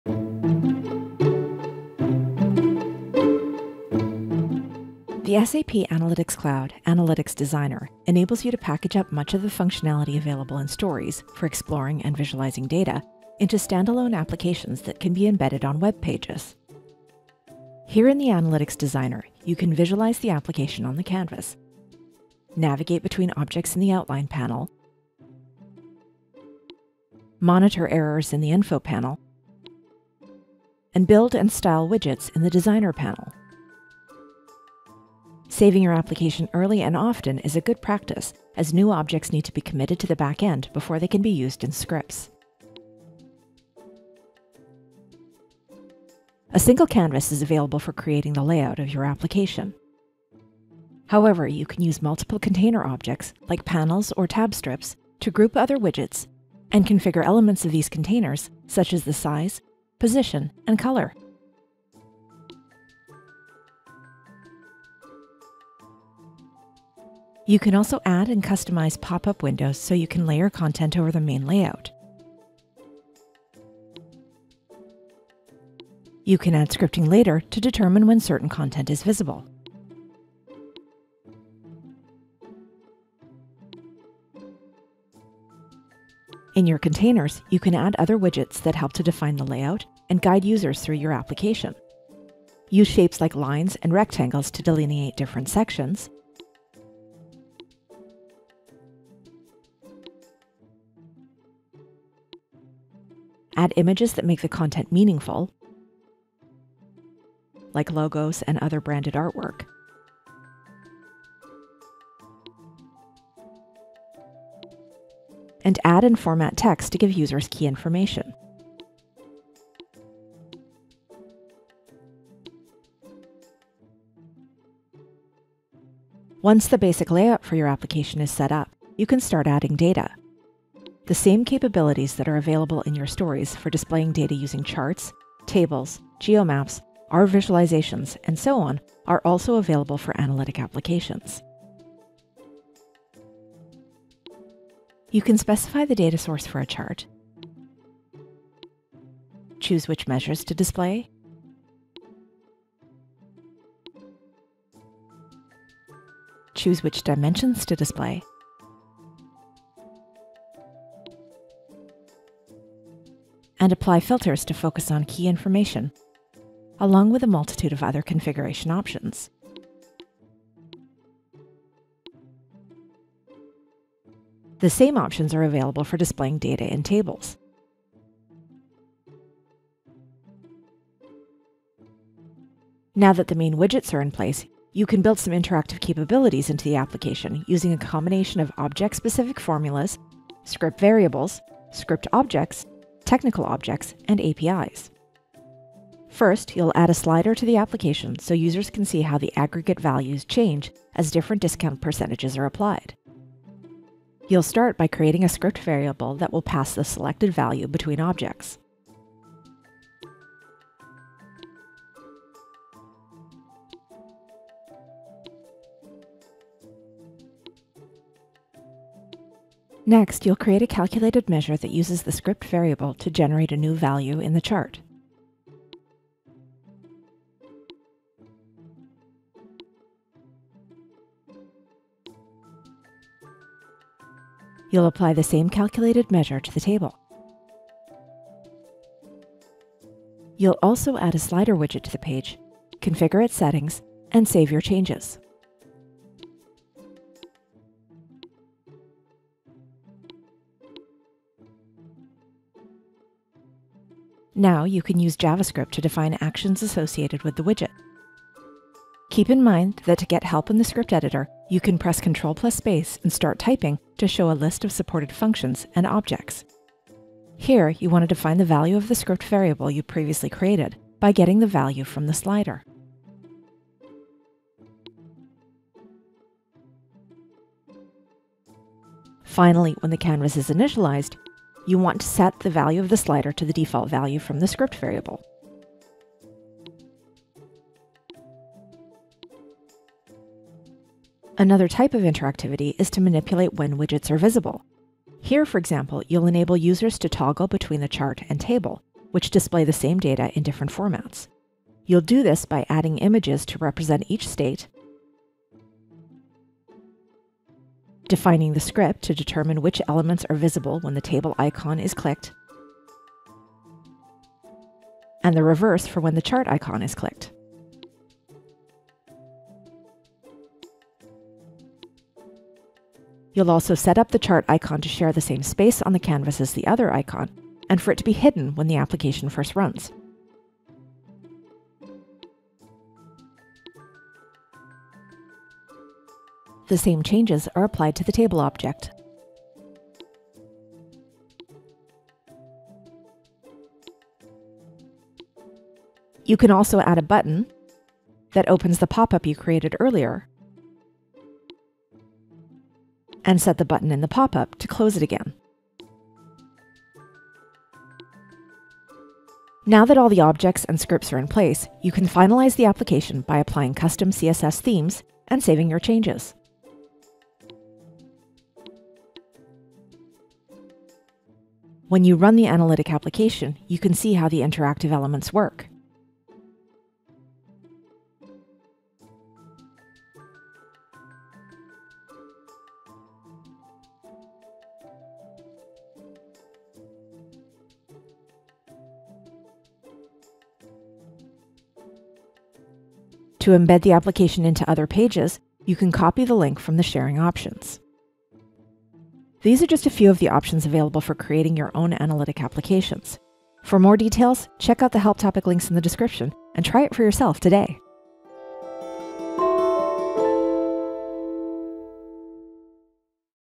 How to Create Analytic Applications: SAP Analytics Cloud
In this video, you will create an analytic application and review some of the options available for visualizing data and building interactivity through scripting.

Chapters:
0:00 Create a new analytic application
0:45 Set up the layout
2:04 Visualize data in charts and tables
3:15 Add a slider to change values
4:50 Display a popup window
6:12 Run the analytic application